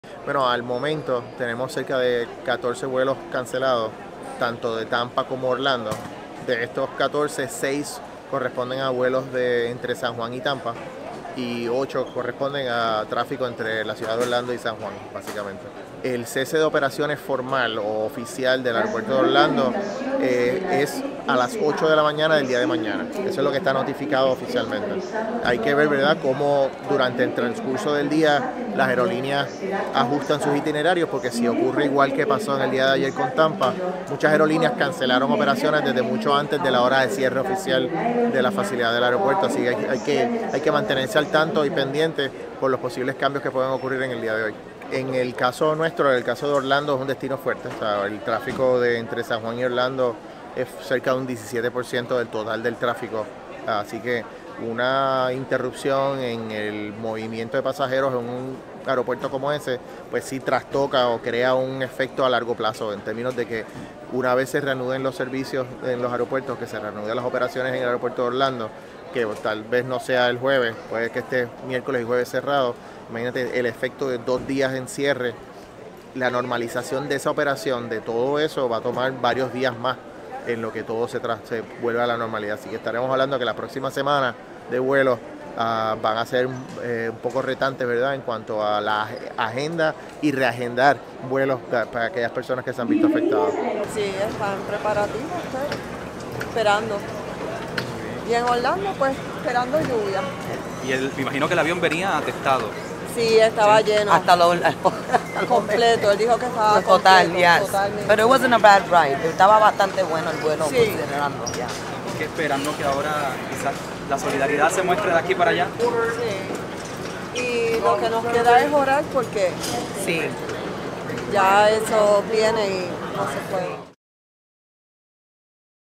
“Lo que nos queda es orar”: así llegan al aeropuerto Luis Muñoz Marín por el huracán Milton
Aerostar explica cómo el impacto ciclónico en Florida afecta las operaciones del principal puerto aéreo de Puerto Rico.